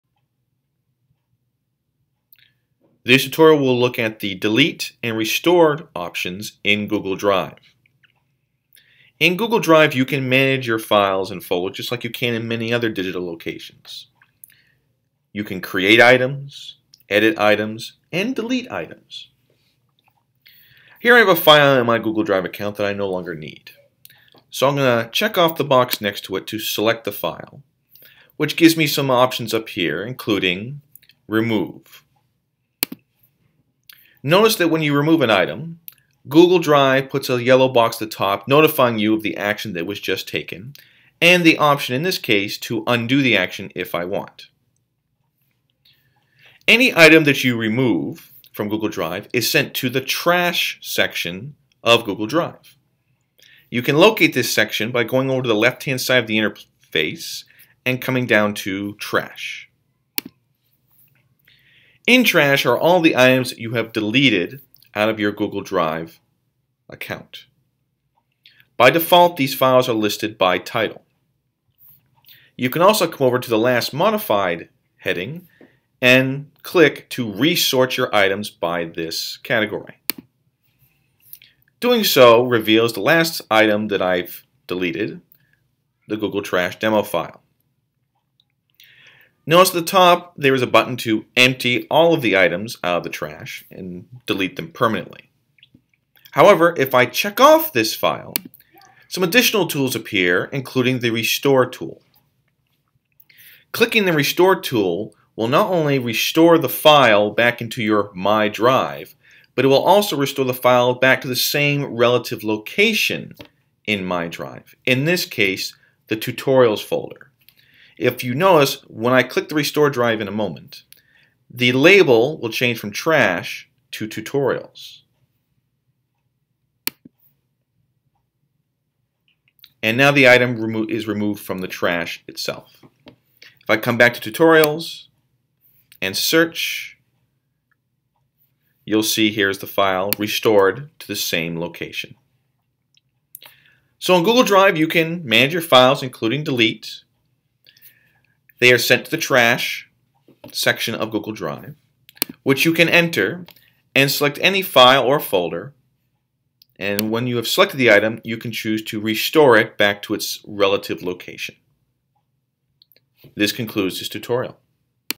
HowTo - Delete & Restore in Drive
This tutorial will show how the Remove and Restore tools work in Google Drive.